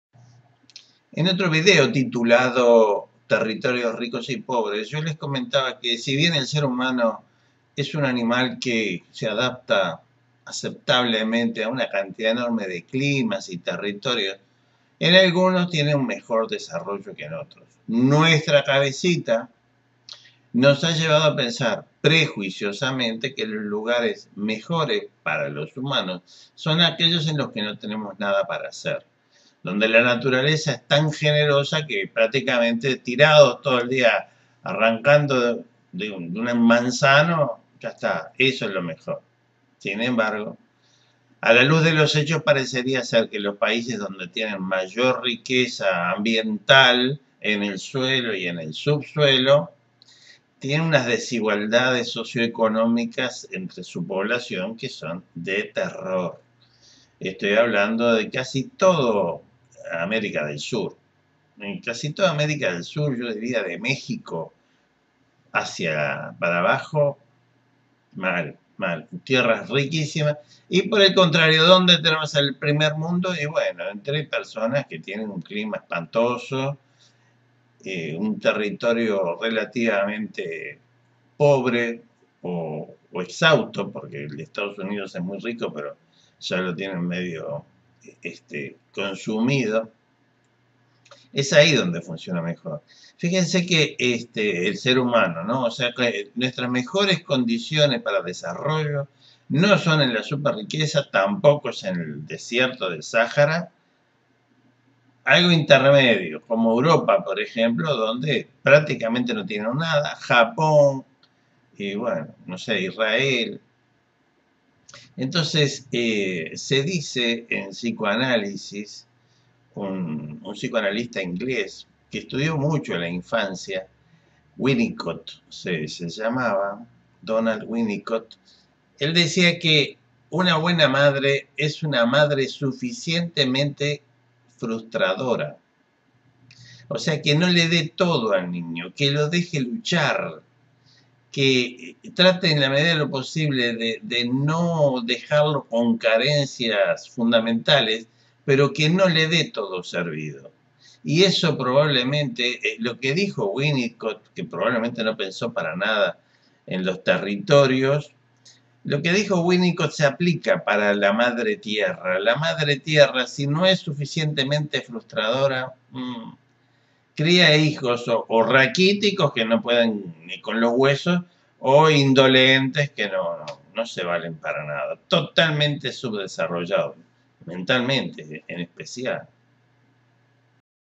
Territorio suficientemente frustrador
Así como las madres ayudan a sus hijos no sobreprotegiéndolos, los 'buenos países' no deberían sobreproteger a sus ciudadanos. Este video está vinculado a un artículo que se publica